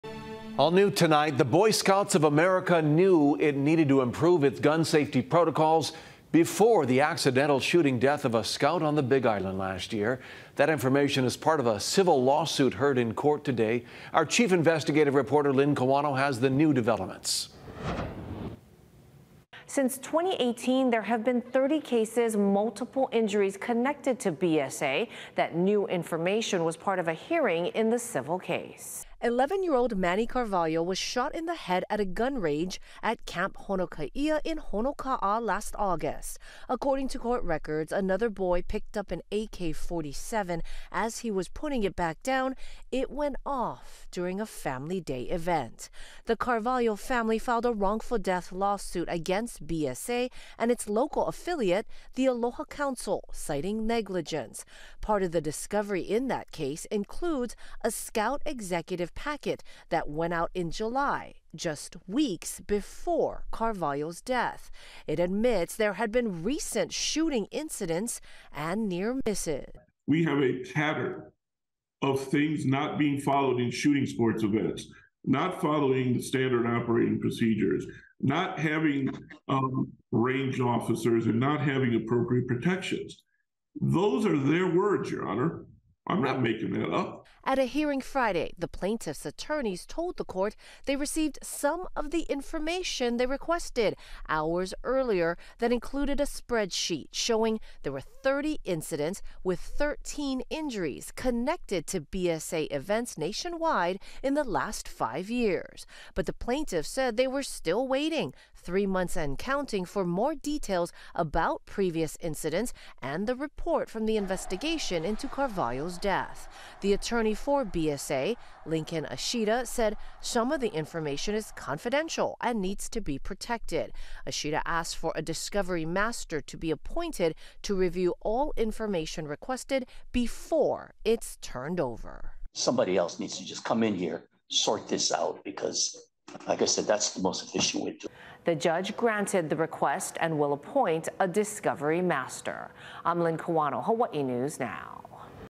Boy Scouts admit 30 shooting probes, near-misses prior to Big Island scout’s death
The new information was included in a memo unearthed as part of discovery in a wrongful death suit.